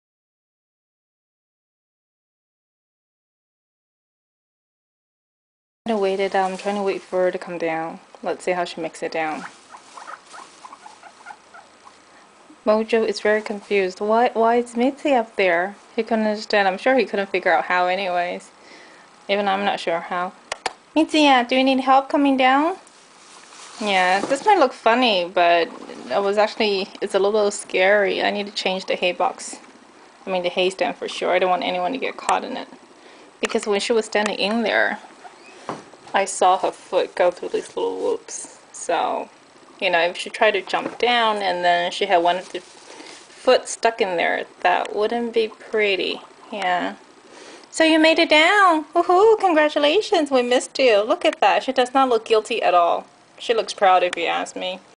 mitzy UP
I think Mitzy tagged along when we went to watch UP the movie...
This is how I found her when I came home after taking down the trash.
Not 2 minutes in between and she has claimed the entire rack hers!  :)

*no piggies was harmed in the making of this video